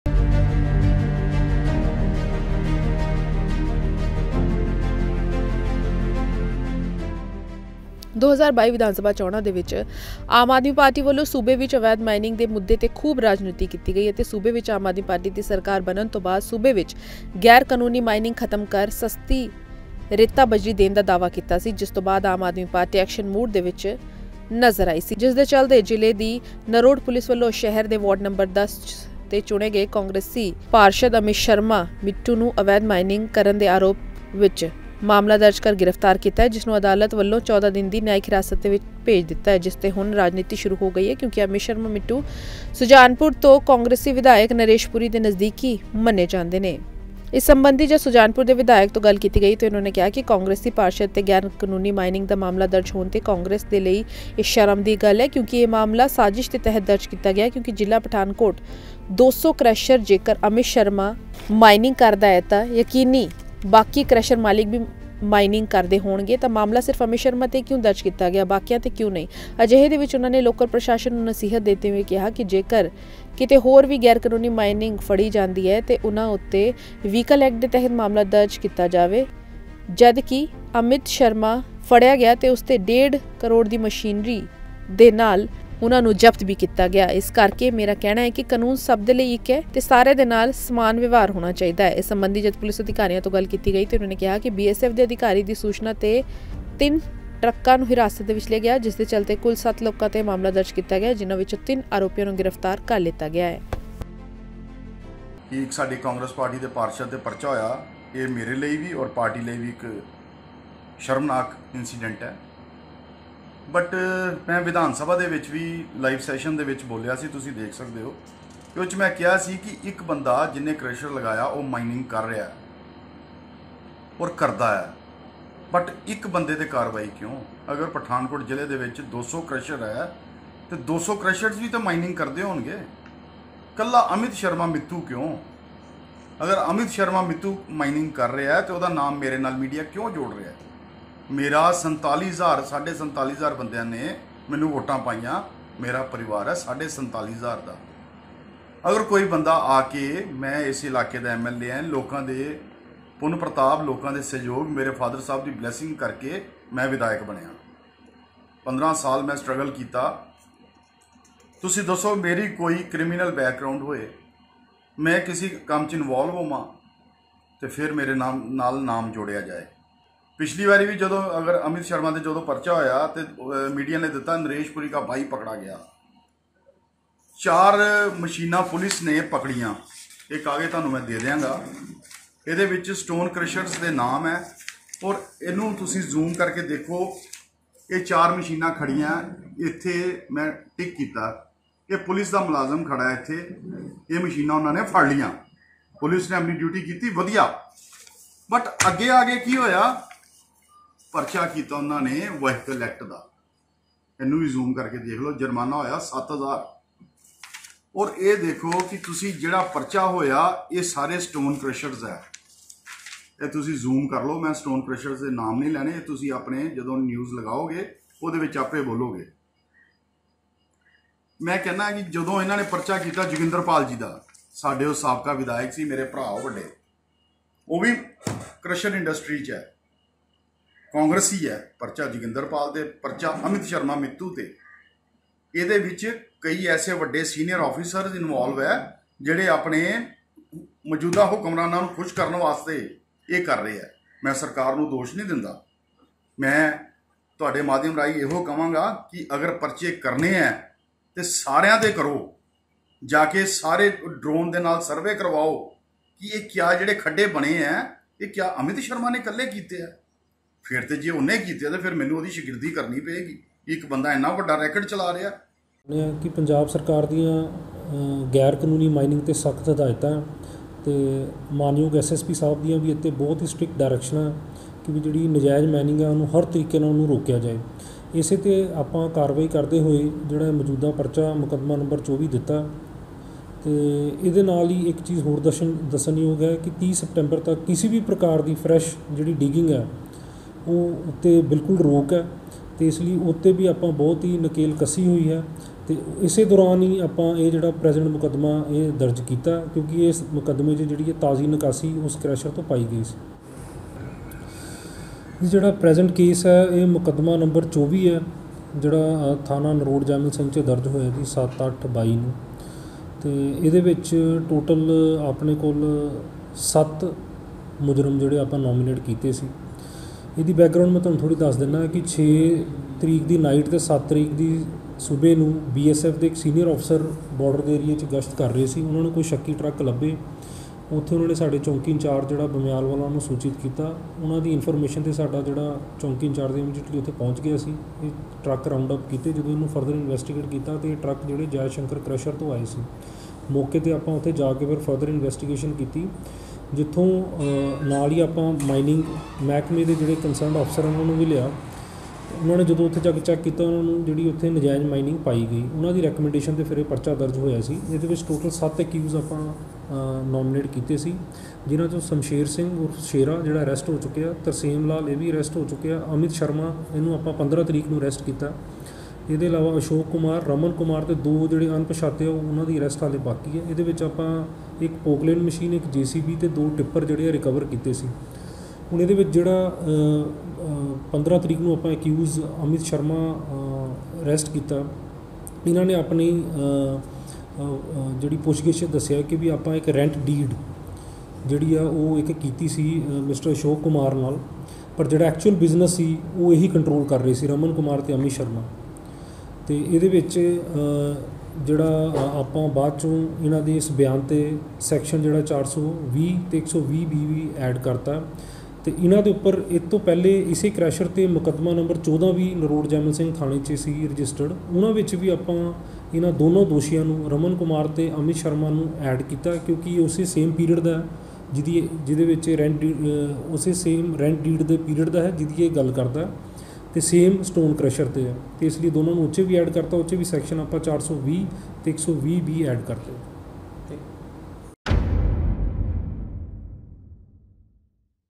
Congress councilor Amit Sharma Mittu arrested in illegal mining case| Punjab Today
ਨਾਜਾਇਜ਼ ਮਾਈਨਿੰਗ ਮਾਮਲੇ 'ਚ ਕਾਂਗਰਸੀ ਕੌਂਸਲਰ ਅਮਿਤ ਸ਼ਰਮਾ ਮਿੱਠੂ ਗ੍ਰਿਫਤਾਰ